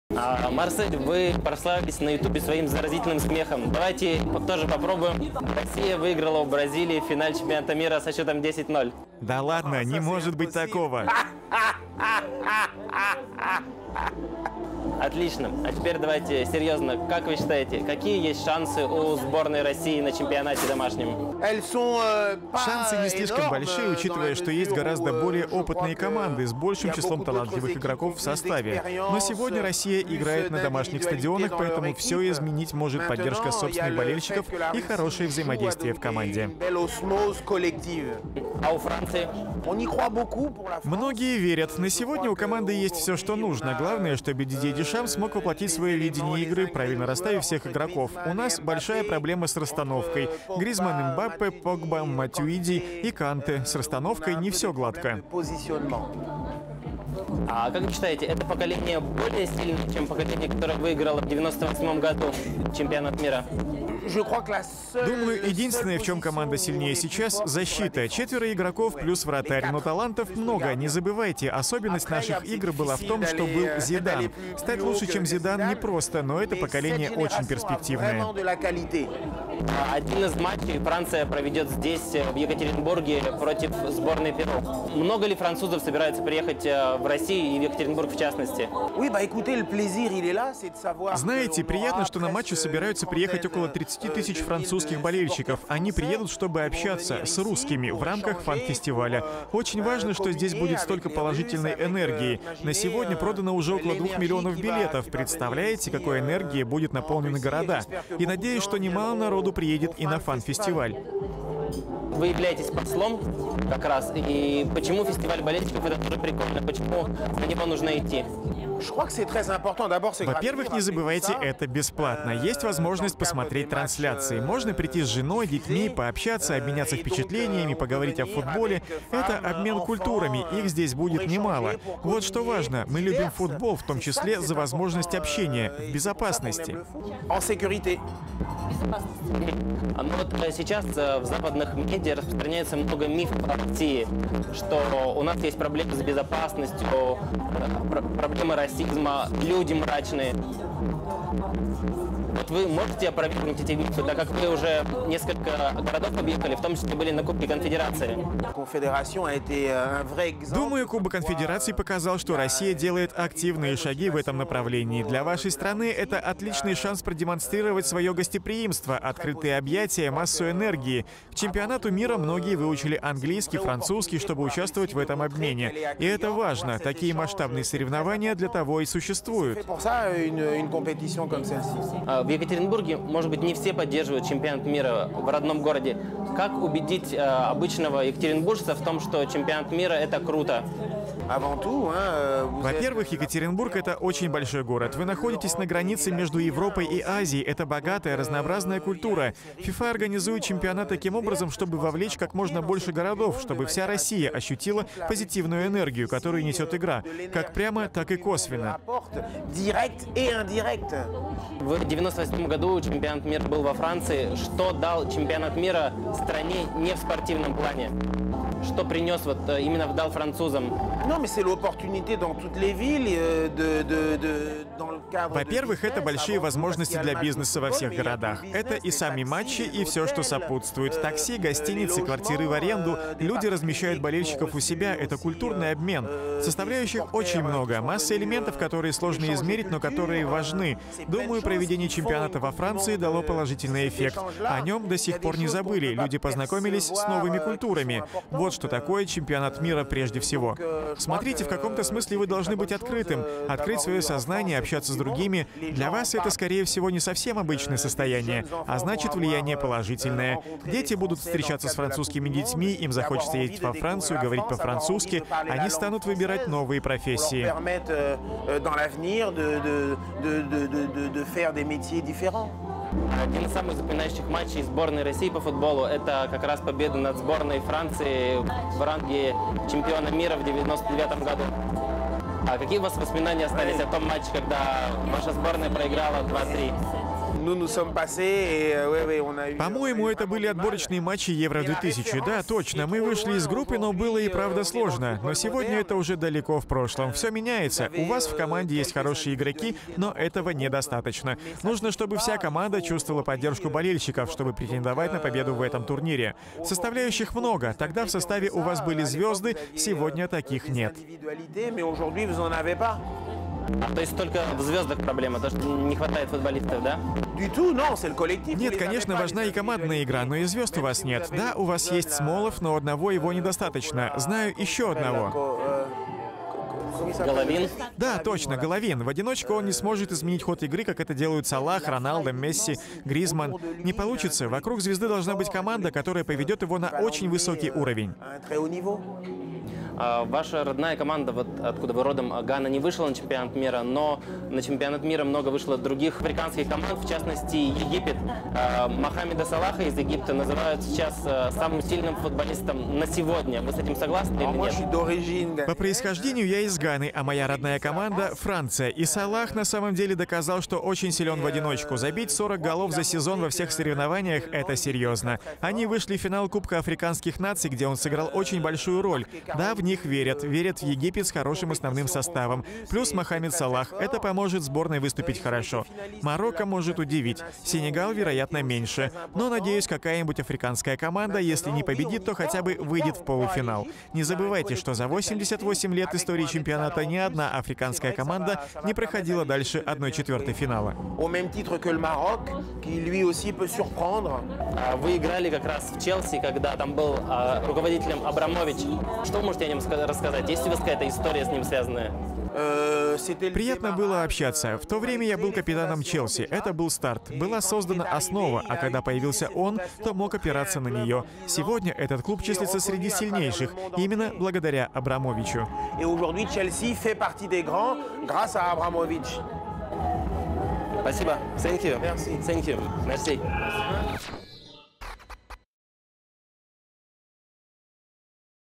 Марсель Десайи. Большое интервью
Чемпион мира и Европы по футболу, защитник сборной Франции Марсель Десайи о ЧМ-2018 в России, Зидане, Федоре Смолове и Романе Абрамовиче.